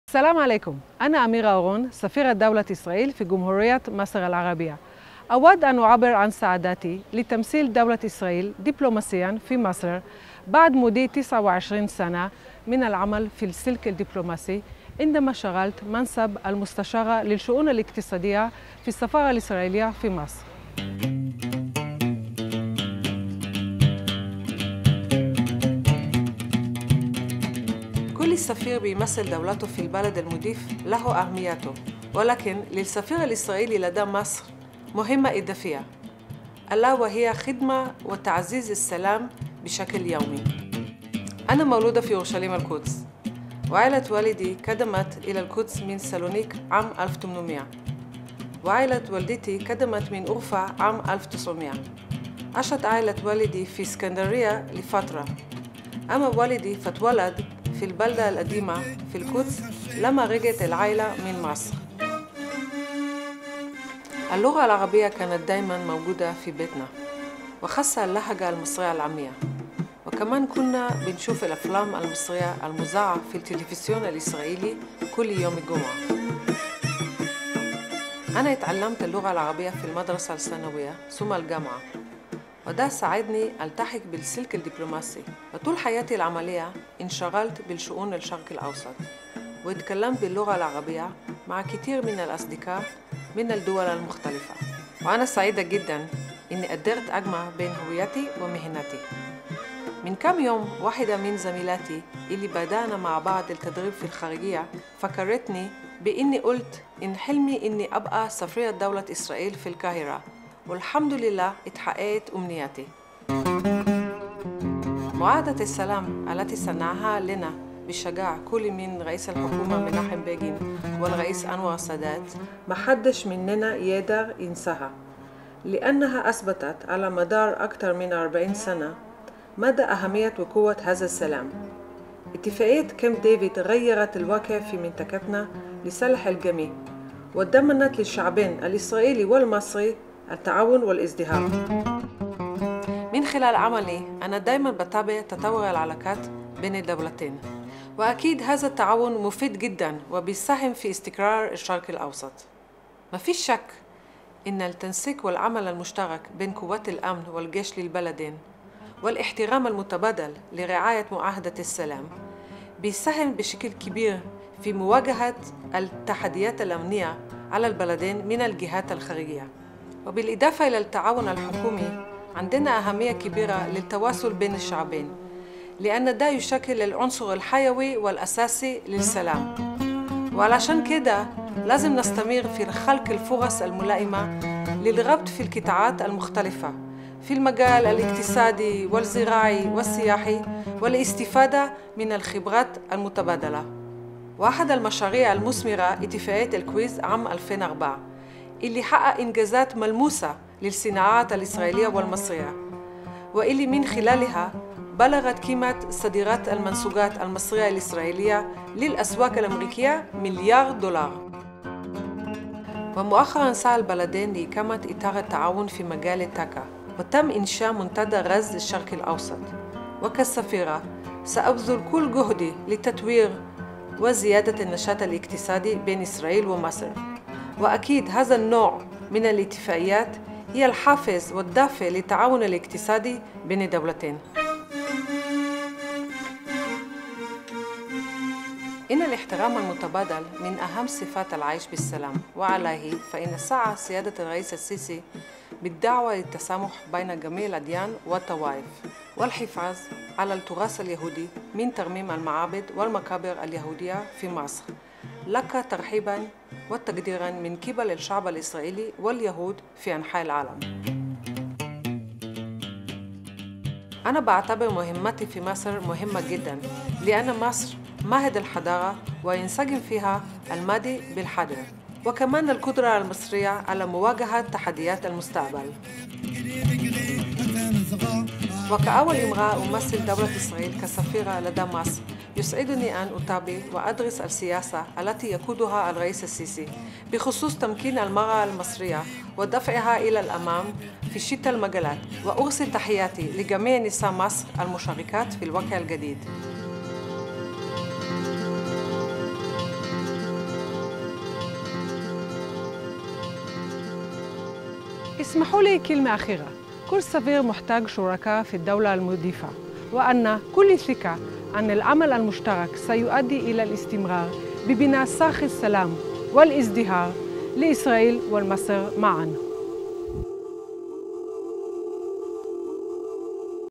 Meet Israel's New Ambassador to Egypt- Amira Oron
Ambassador Oron is a veteran diplomat who has committed herself to the foreign service for over 29 years.

Her newest posting in Egypt is particularly meaningful. Ambassador Oron is the first woman to assume the role of Israel's ambassador to Egypt.

We wish Ambassador Ben Zvi much success in her new role and in strengthening ties between Israel and Egypt 🇮🇱 🇪🇬